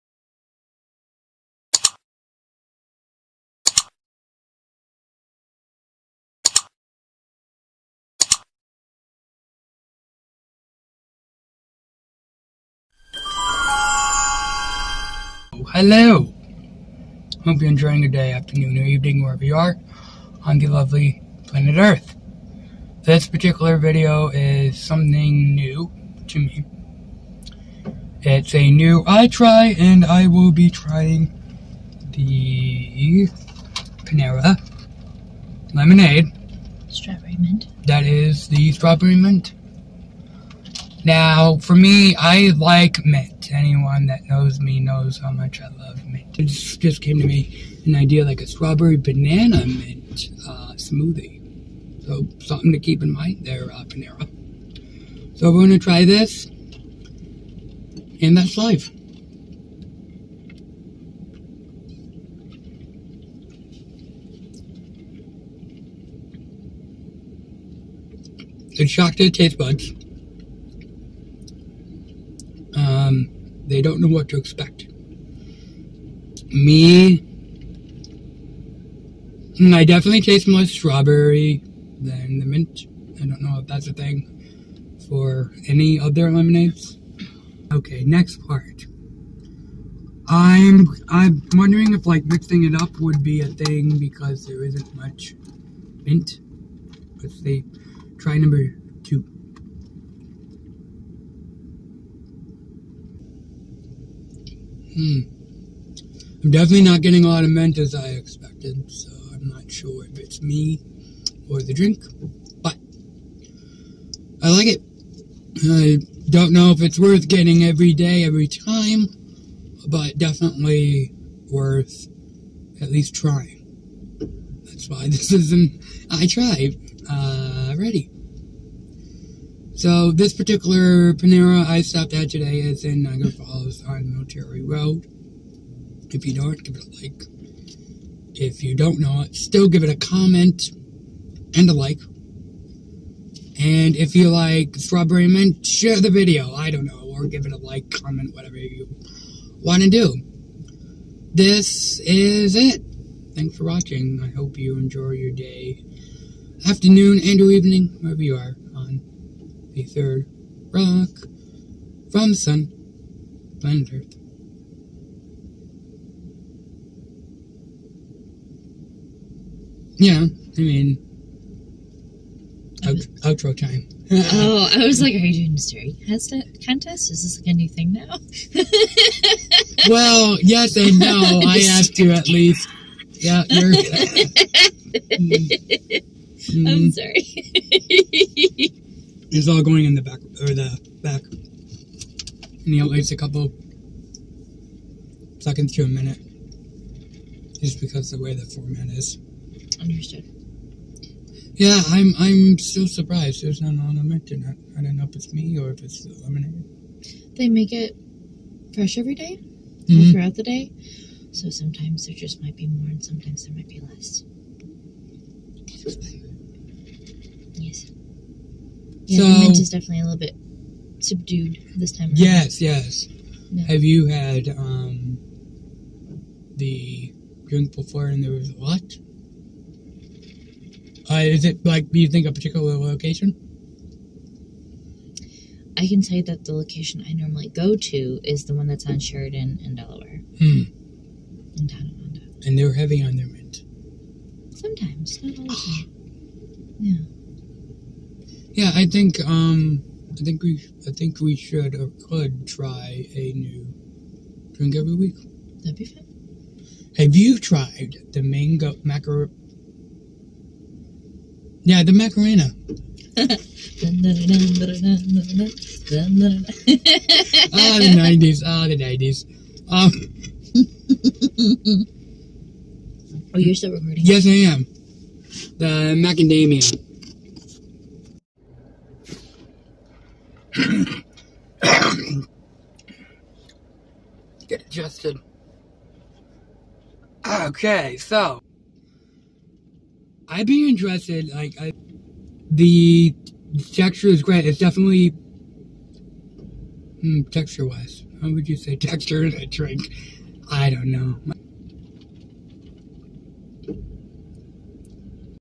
DavidsLife vlog blog WNY itry panerabread lemonade
Hello everyone! I tried something new, found it at panerabread! So you'll mostl likely thing alright what is it? The drink iTry was StrawberryMint lemonade! First for me trying this kind of lemonade and lemonade as a whole!

Thanks for stopping by my little slice of the internet.

Almost forgot! My blog is up, live, and active! This is where all my non YouTube life is